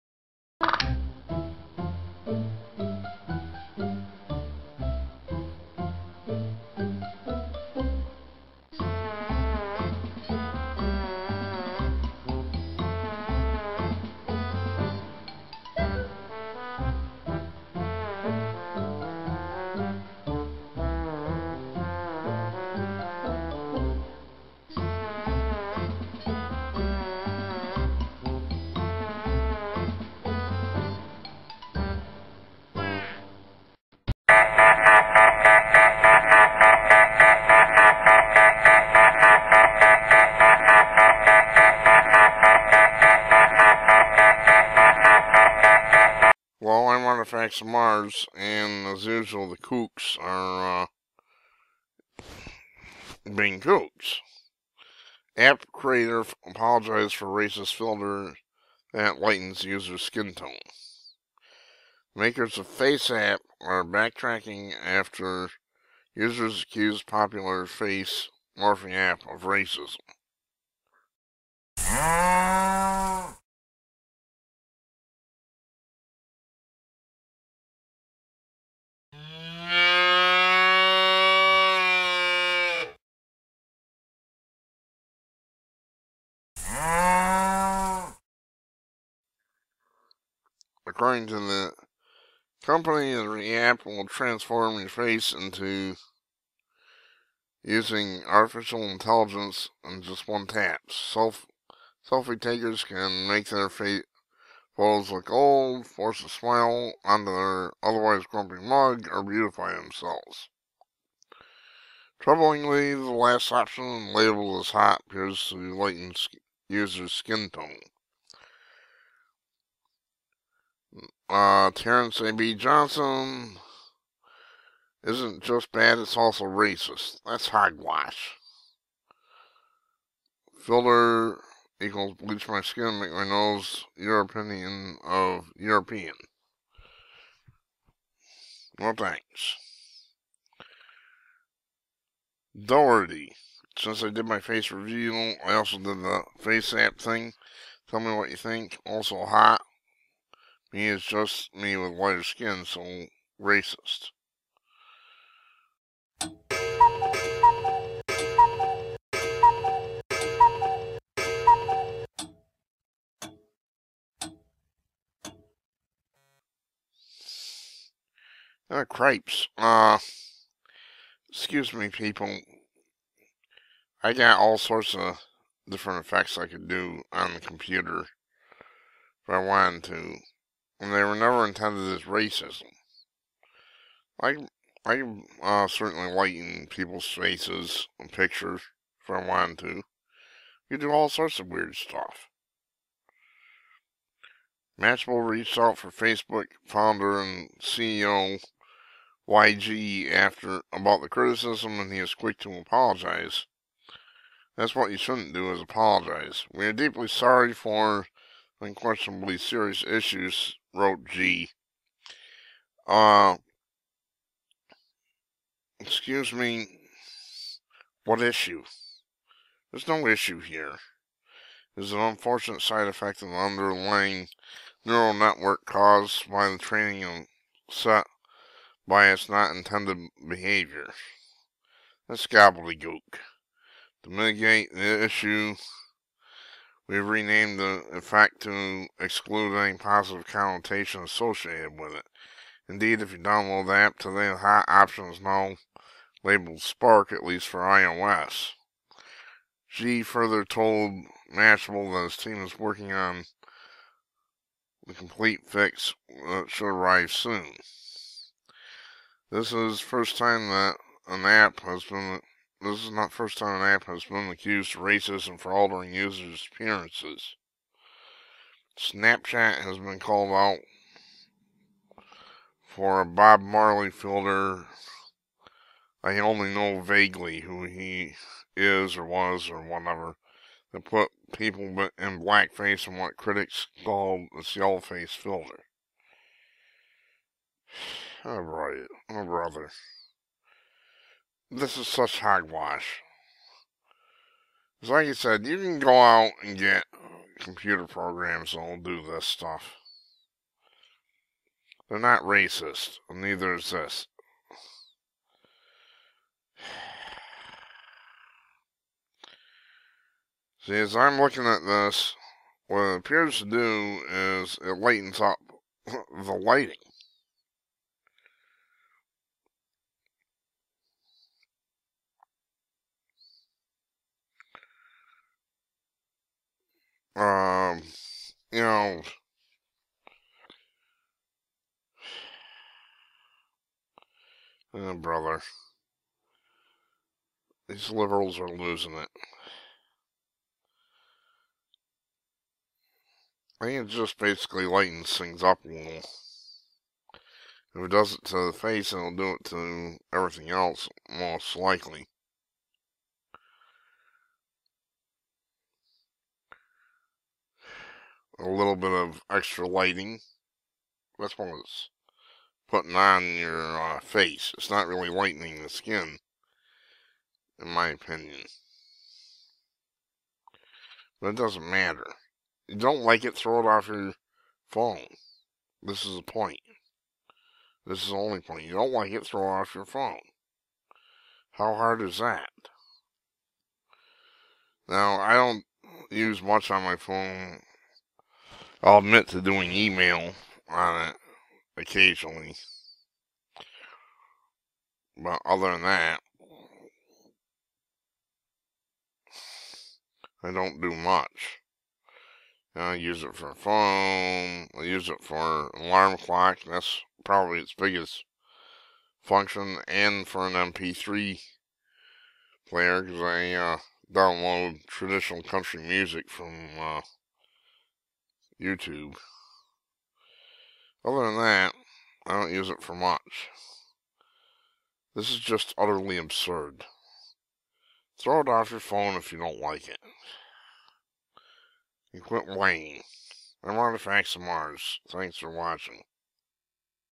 Fingers pointed at app maker for face lightening app
Faceapp, a cell phone app, has a function called "hot" which apparently lightens skin tone. This got liberals all upset and now the app is being changed.